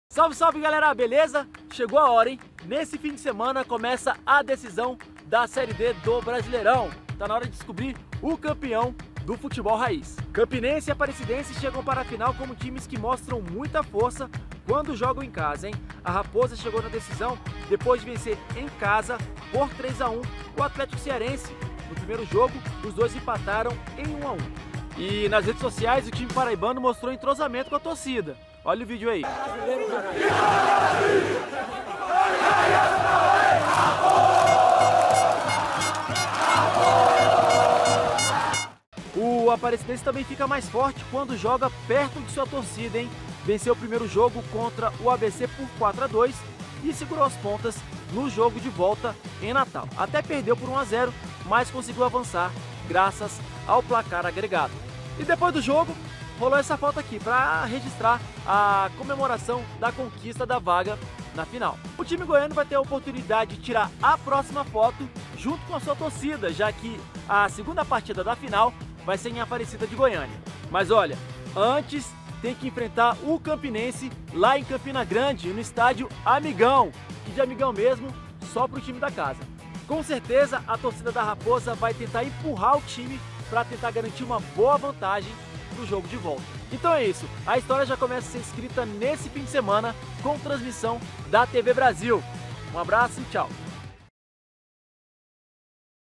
SérieD | Primeiro jogo da final
Neste fim de semana, o Campinense enfrenta o Aparecidense pela final da Série D do Brasileirão! A @TVBrasil exibe o jogo no sábado (06/10), ao vivo, às 16h.